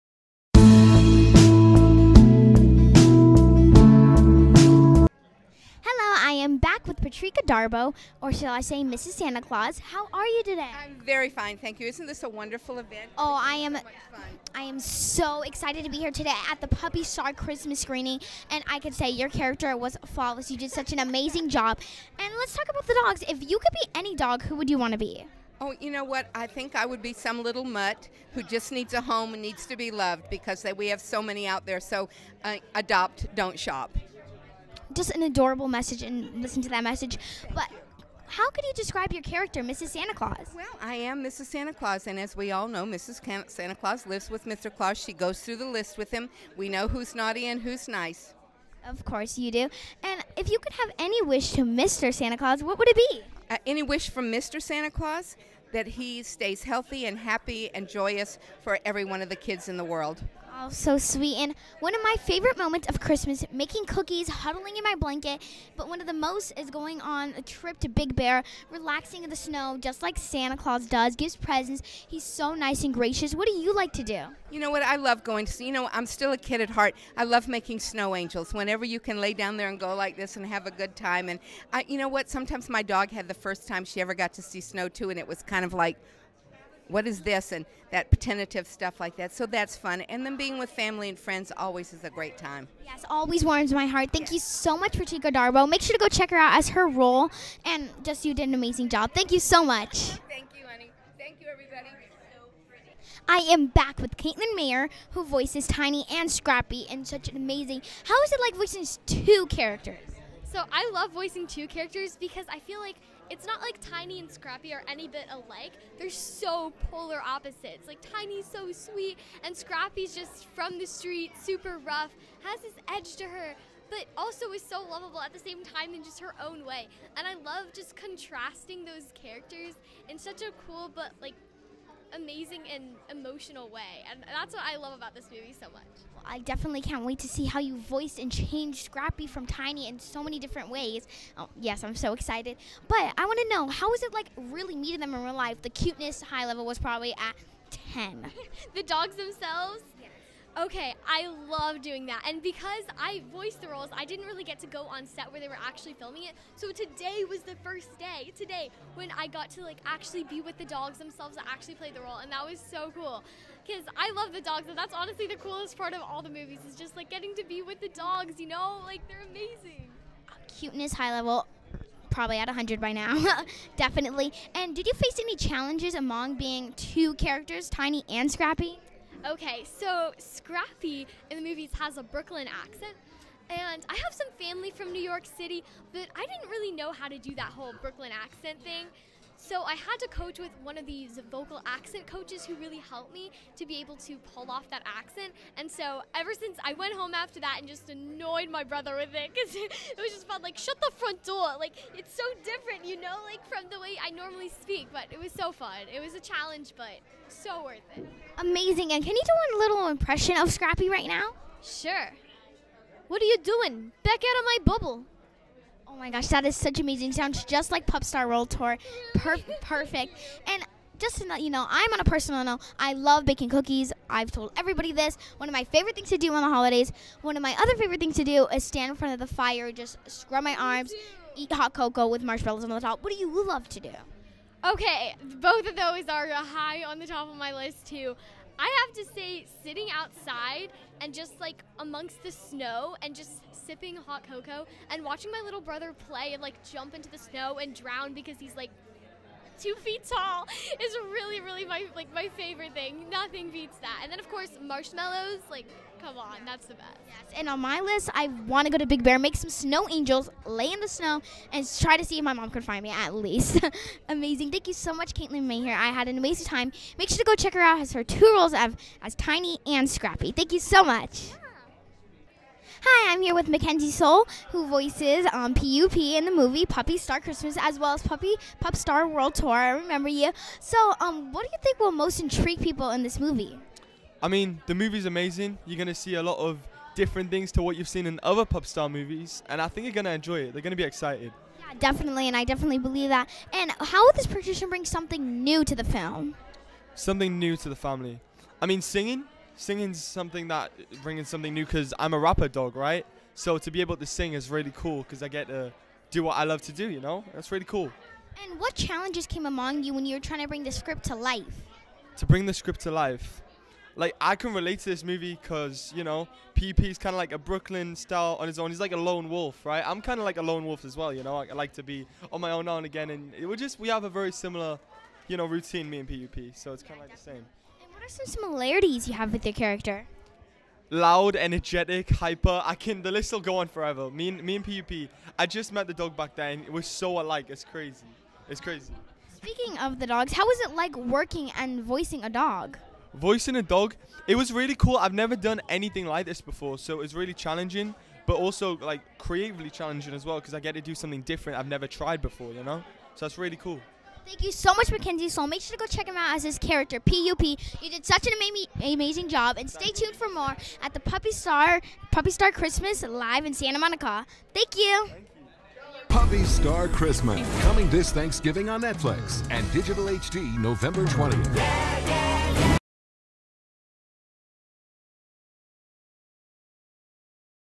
Interviews with Makenzie Moss, Makenzie Sol and mroe from Puppy Star Christmas by Nathalia J.
Interviews with Patrika Darbo,  Kaitlyn Maher, Makenzie Moss and Makenzie Sol from Puppy Star Christmas by Nathalia J. Directed by Robert Vince; Writers: Piers Rae, Robert Vince; Starring Diedrich Bader, George Newbern, Richard Riehle, Makenzie Solo, Makenzie Moss and more. Available November 20, 2018 on Netflix.